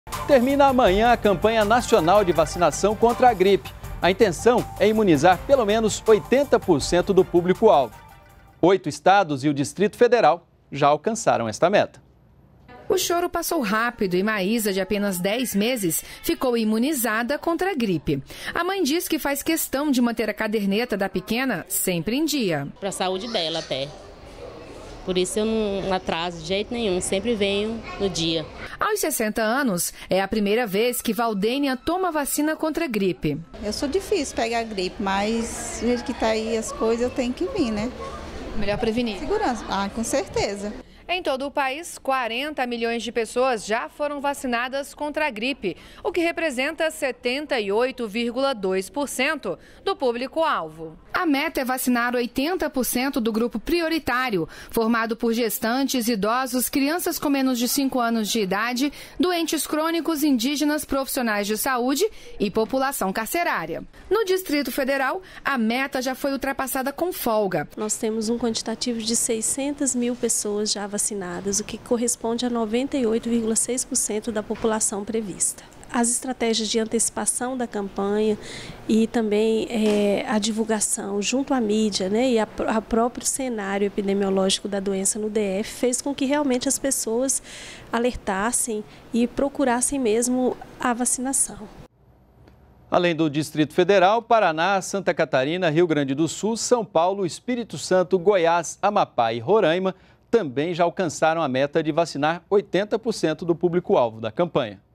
Campanha contra gripe termina nesta sexta
JORNAL NBR - 19.05.16: Termina nesta sexta-feira (20) a Campanha Nacional de Vacinação contra a Gripe. A intenção do governo é imunizar 80% do público-alvo. Oito estados e o Distrito Federal, já alcançaram essa meta.